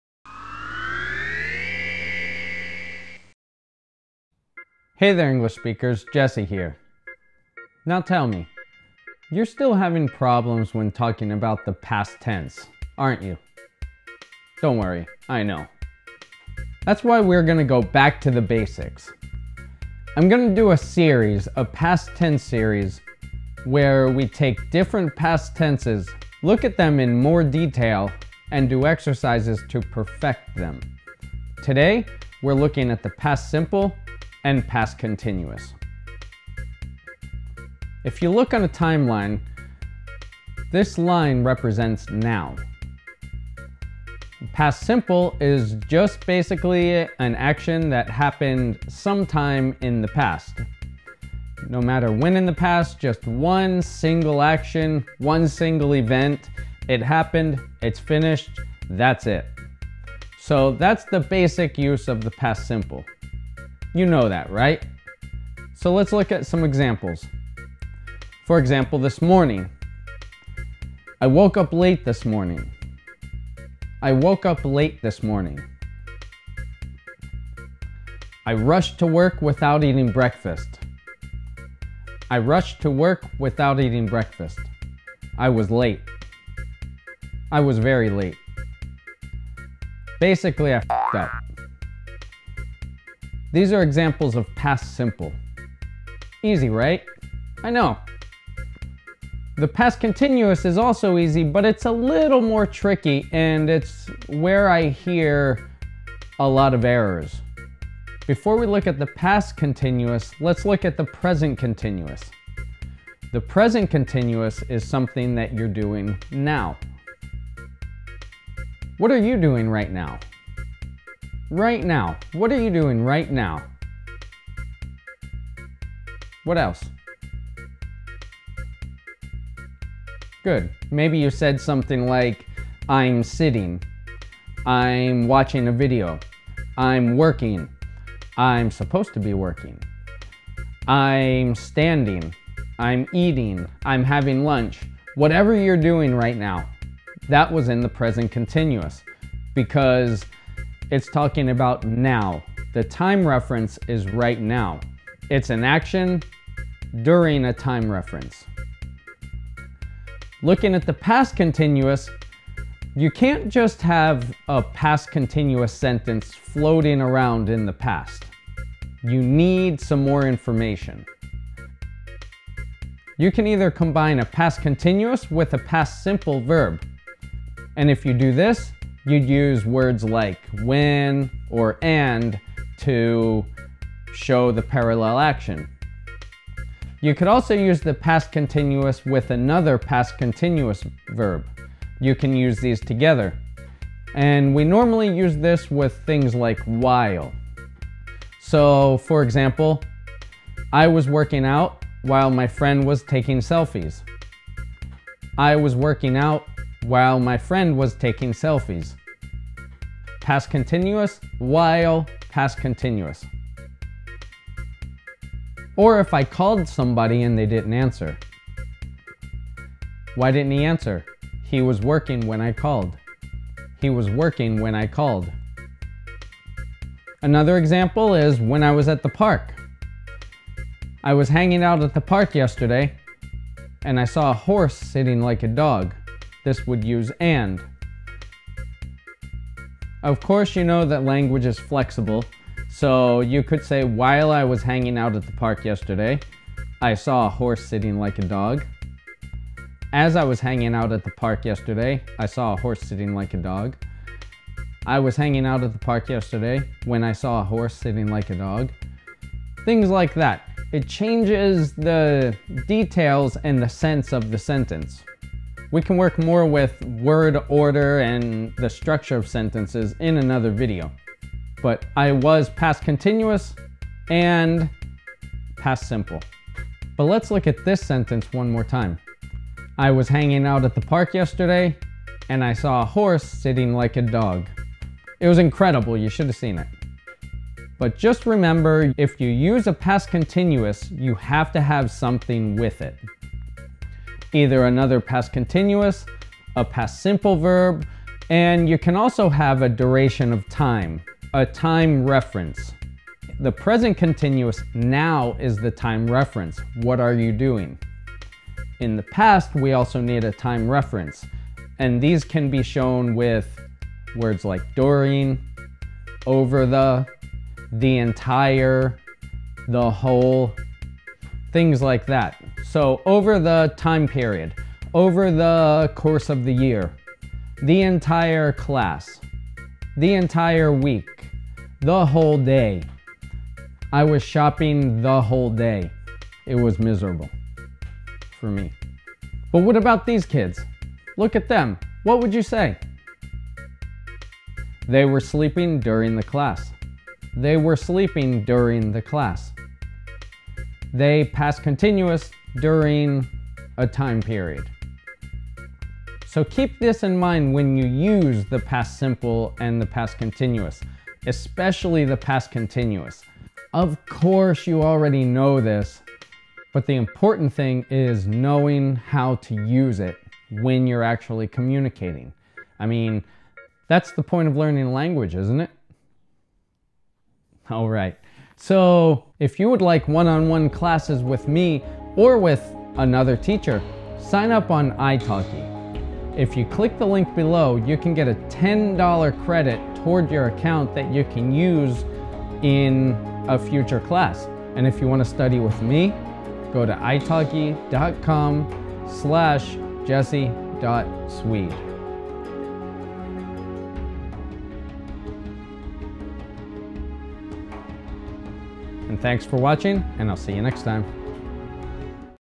Back to Basics: Past Simple and Past Continuous (Intermediate Lesson)
Are you still making mistakes when talking about the past when speaking or writing?

Our Back to Basics series is intended to practice and perfect the different past tenses so that you can eliminate those errors for good!

Today, we will learn the Past Simple and Past Continuous with grammar, examples, and exercises.

languagegalaxy.net

Facebook.com/LanguageGalaxy
Twitter.com/Jesse_Galaxy